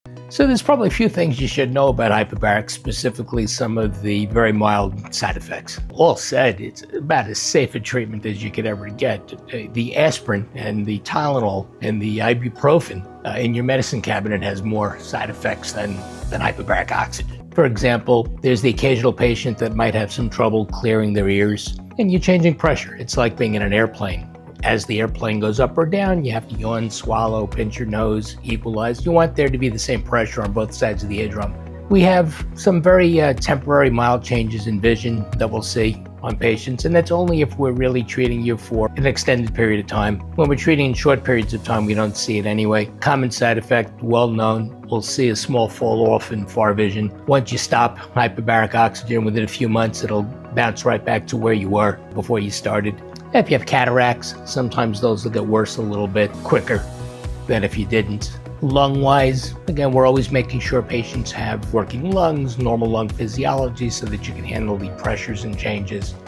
Is HBOT Safe? (Hyperbaric Oxygen Therapy)
Is HBOT Safe? ✅ Yes—when delivered in the right medical environment.

Hyperbaric Oxygen Therapy (HBOT) is a safe, non-invasive treatment with minimal side effects and significant health benefits—but only when administered correctly.

At Hyperbaric Medical Solutions, safety is our top priority.
Here’s what sets us apart:
✅ Treatments are overseen by board-certified physicians
✅ All chambers are FDA-approved and medical grade
✅ Sessions are operated by certified hyperbaric technicians
✅ Strict safety protocols are followed at every step

Possible side effects include:
✨ Mild ear pressure (like flying), relieved by yawning or swallowing
✨ Temporary vision changes with extended treatment (usually resolve within months)
✨ Rarely, oxygen toxicity can cause minor twitching—easily managed by adjusting oxygen levels

When done properly, HBOT is safer than many common medications like aspirin or ibuprofen.